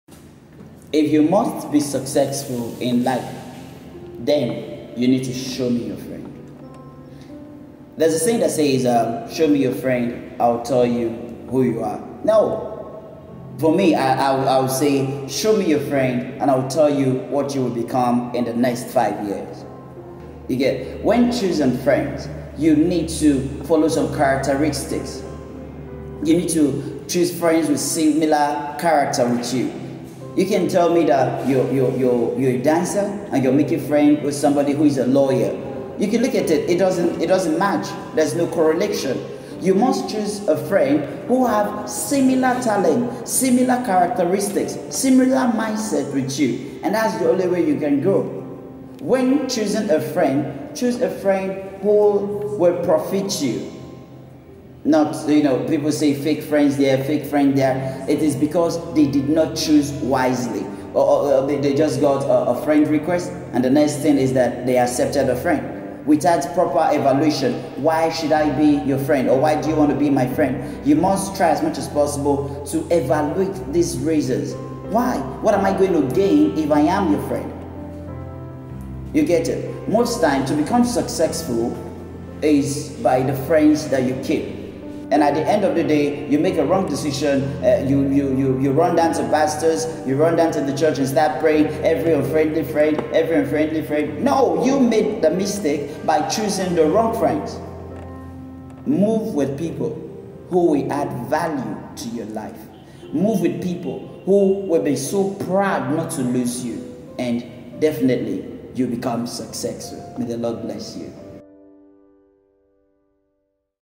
Show me your friend by Omofuma Ernest. make a good choice when chosing friends.
chose friends that will add value to you.